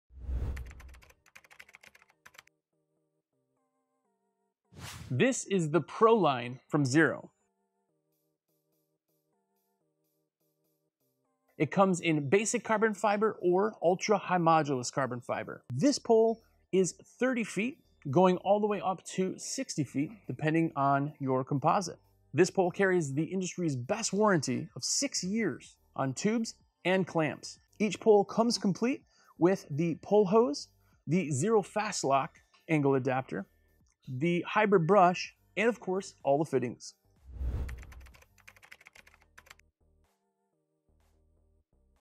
The XERO Pro Series
The XERO Pro Water Fed Pole series includes some of the most affordable carbon fiber poles on the market! Available in two models: Pro Basic and Ultra-Light High Mod. Perfect for residential or commercial jobs!